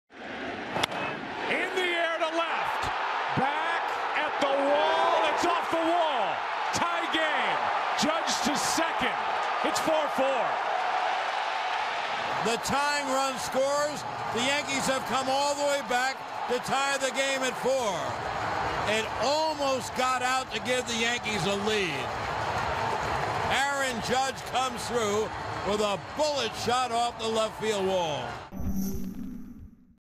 ALCS Gm4: Judge ties game with an RBI double in 8th
10/17/17: Aaron Judge belts a double to the wall in left, scoring Jacoby Ellsbury to tie the game at 4 in the bottom of the 8th inning in Game 4 of the ALCS